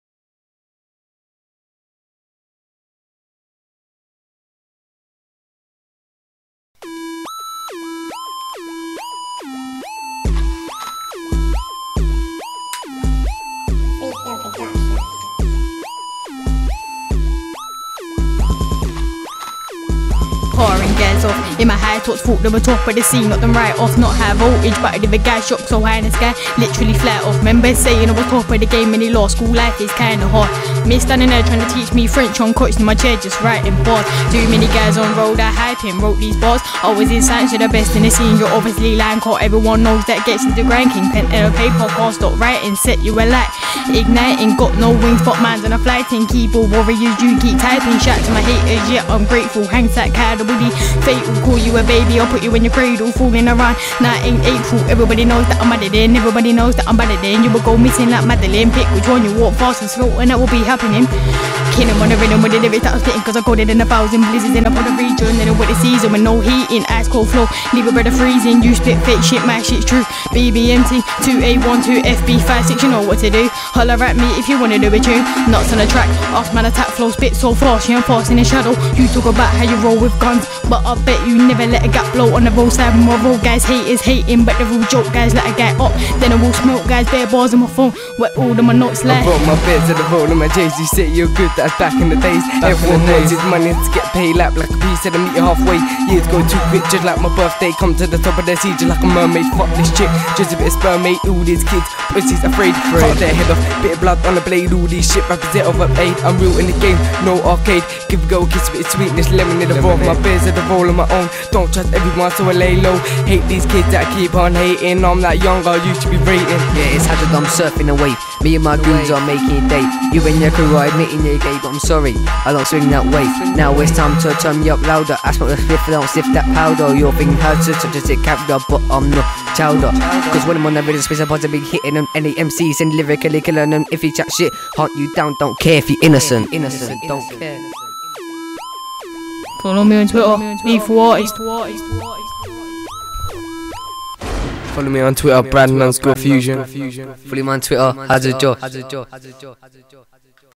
FUSION, HAZZARD & LETHAL [NET VIDEO] [@AXTENFILMS]
Fusion, Hazzard & Lethal performing a quick net video
Directed & Edited by Robert Axten
Follow @AxtenFILMS